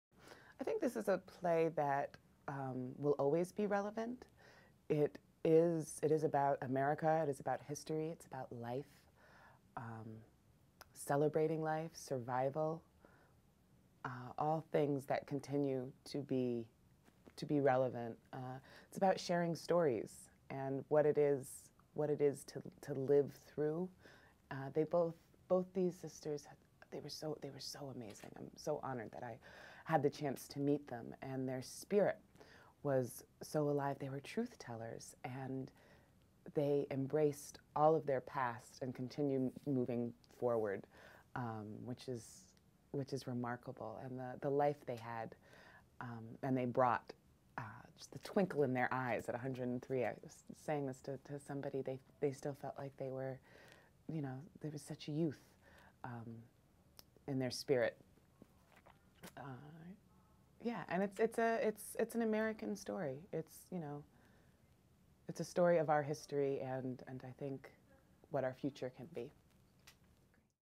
The Relevancy of HAVING OUR SAY Today
Jade King Carroll speaks to the relevancy of the Delany sisters' story and how it resonates with today's audiences.

HAVING OUR SAY:
THE DELANY SISTERS' FIRST 100 YEARS
BY EMILY MANN
ADAPTED FROM THE BOOK BY SARAH L. DELANY AND
A. ELIZABETH DELANY WITH AMY HILL HEARTH
DIRECTED BY JADE KING CARROLL
CLAIRE TOW STAGE IN THE C. NEWTON SCHENCK III THEATRE

Centenarian sisters Sadie and Bessie Delany welcome us into their home to share an amazing story. They tell us about their eventful lives, how they fought injustices big and small, overcoming the racial strife of the 20th century with their charm, warmth, and dignity intact. They share a personal family tale of people who yearned to do the right thing, and strove towards it with every ounce of their beings. Come have dinner with the Delanys, and hear the story of our nation.